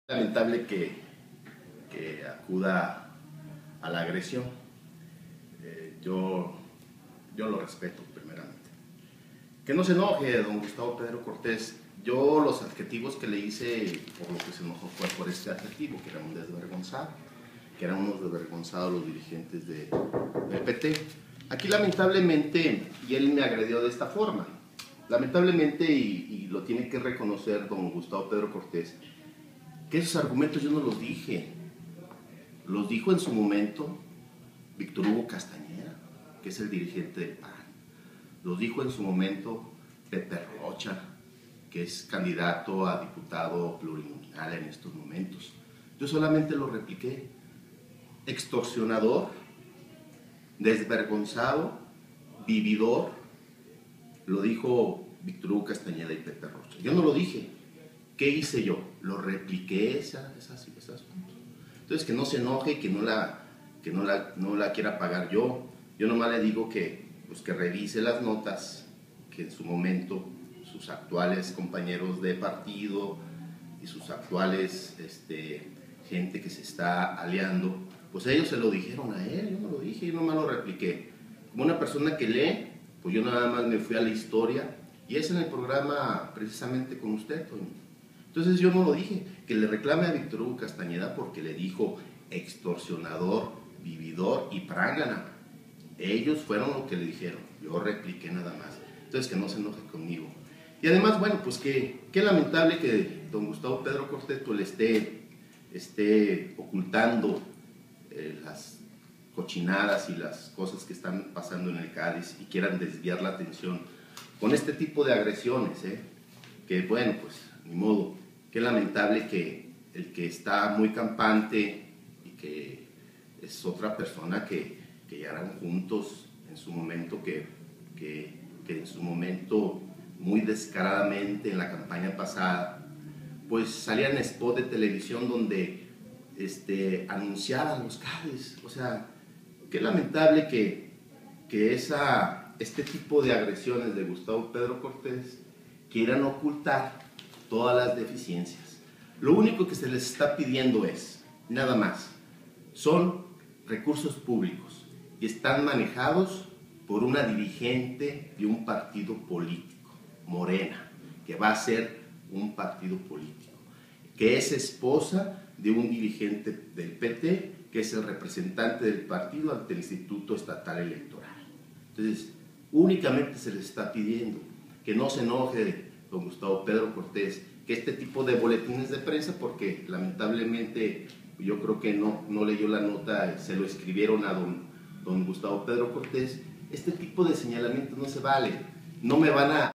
Petistas desvergonzados es una expresión panista y no del PRI: Héctor Partida.
Petistas desvergonzados es una expresión panista y no un adjetivo que surgiera del PRI, reiteró Héctor Partida Romero, vocero del CDE del Partido Revolucionario Institucional, por lo tanto pide al diputado Gustavo Pedro Cortés (PT) reclamé a sus aliados electorales y no a los priistas.